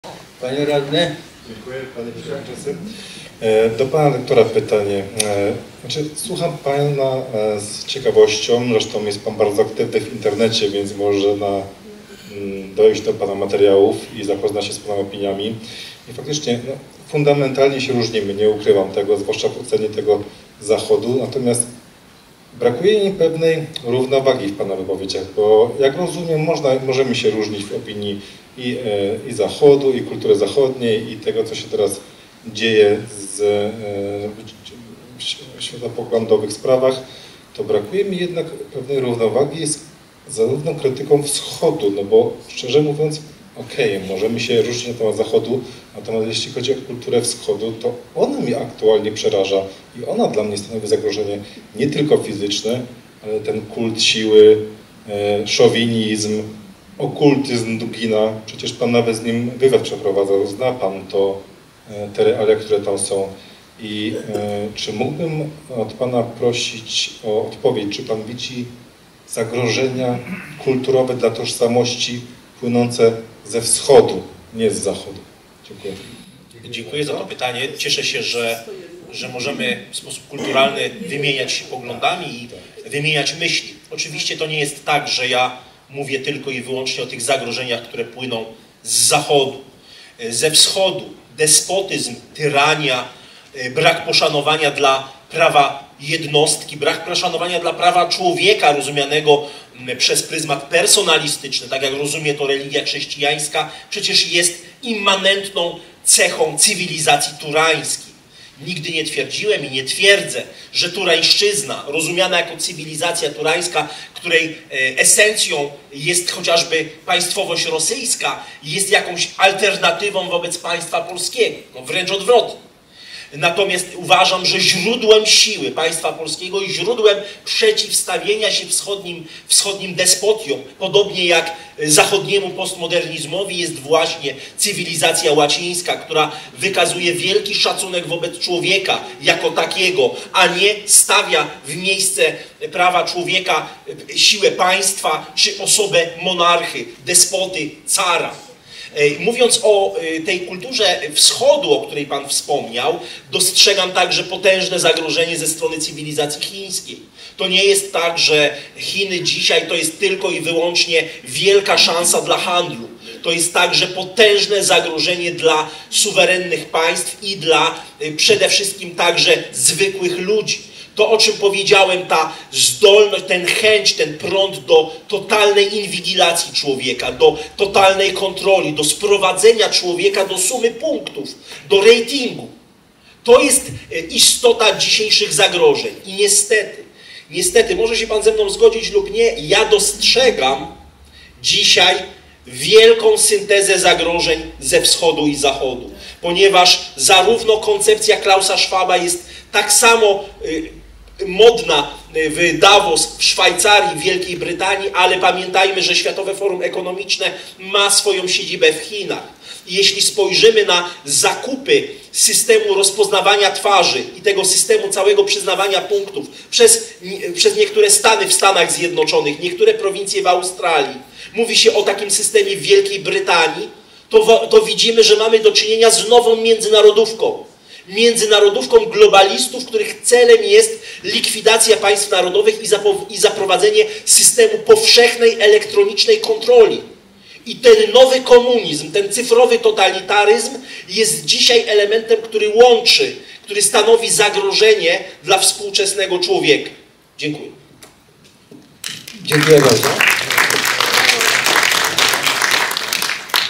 Dr Leszek Sykulski. Niewygodne pytanie z publiczności
22 i 24 października w Tarnowskich Górach odbyło się kolejne Sympozjum Tarnogórskie. Jednym z gości był dr Leszek Sykulski.

Wspieraj naszą działalność, możesz postawić nam wirtualną kawę.